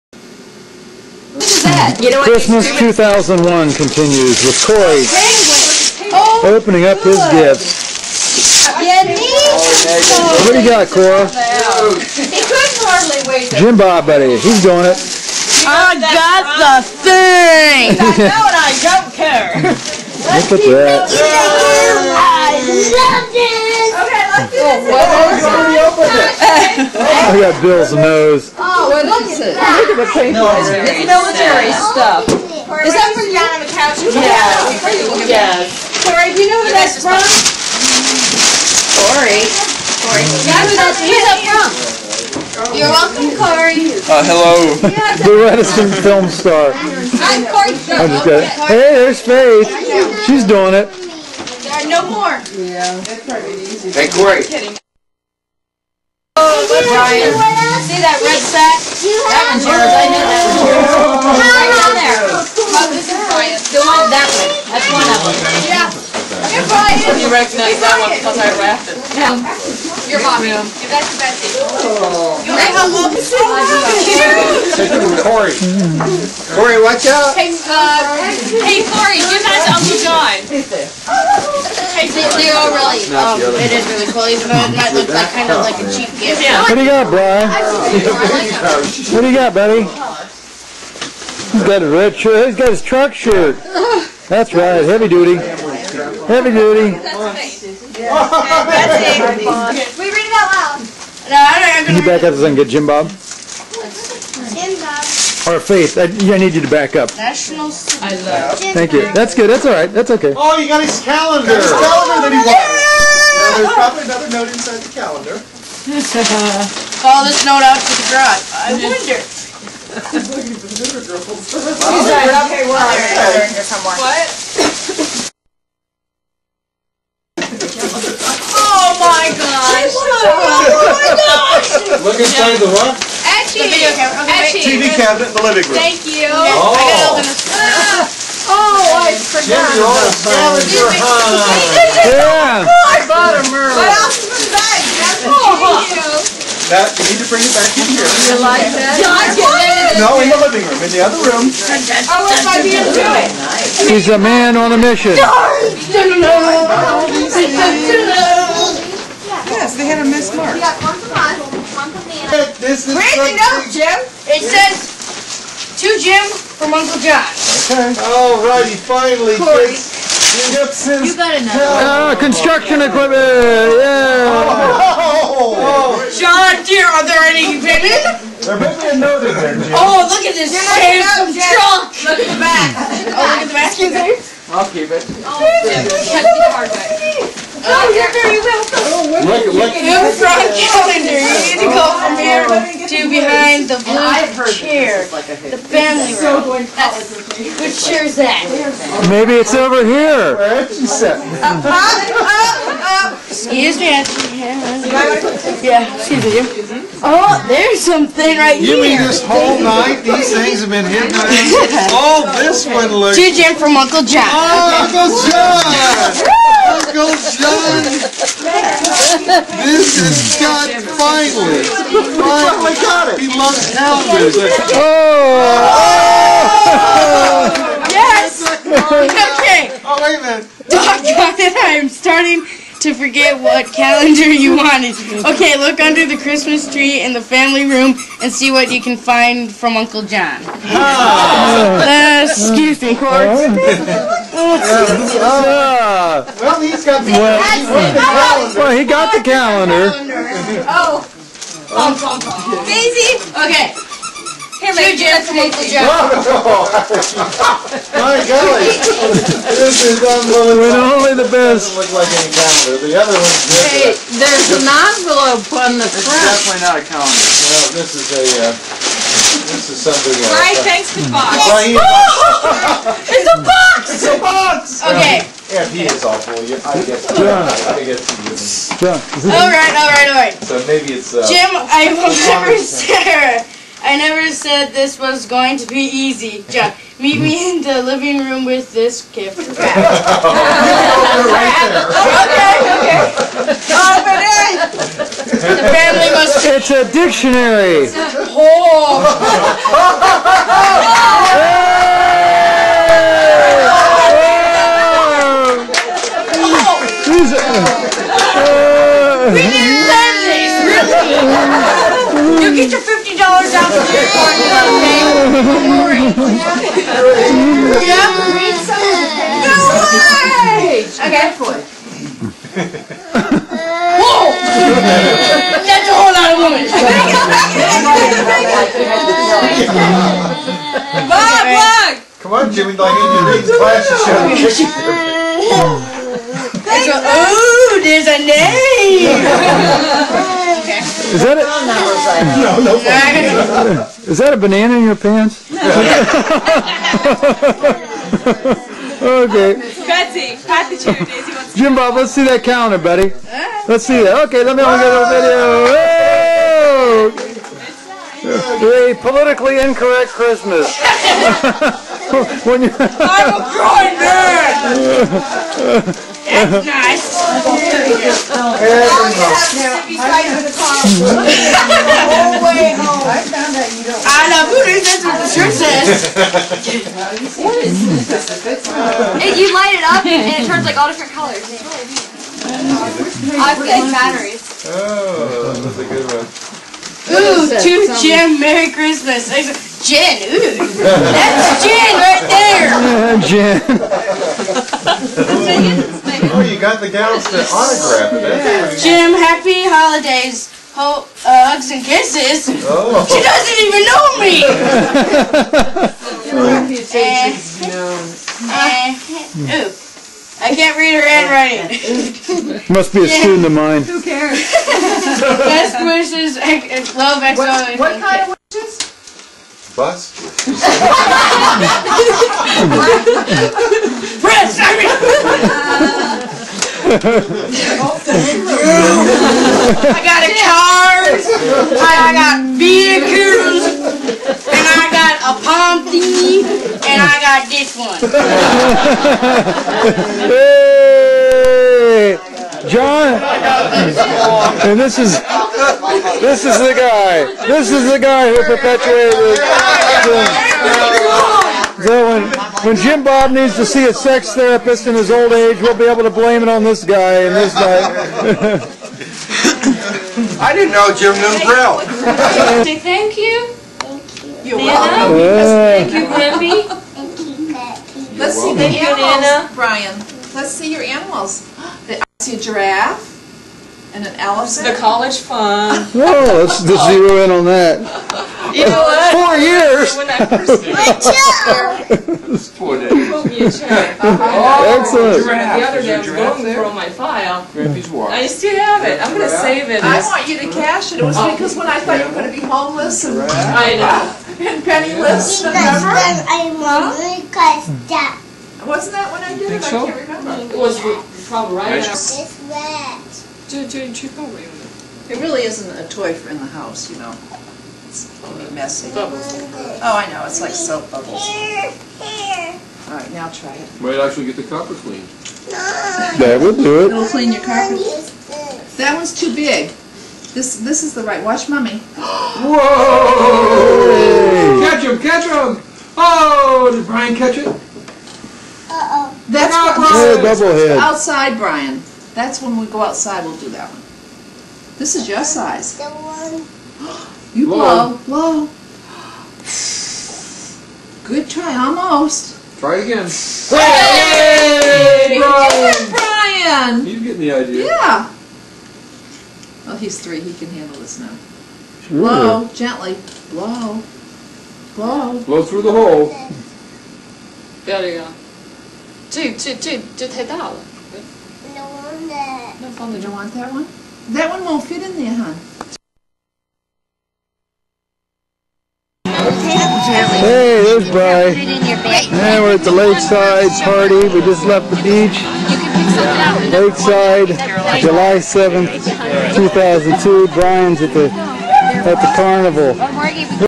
Jim Schnell Nob Hill Schnells "Christmas, 2001"
The Schnells gather for Christmas, 2001.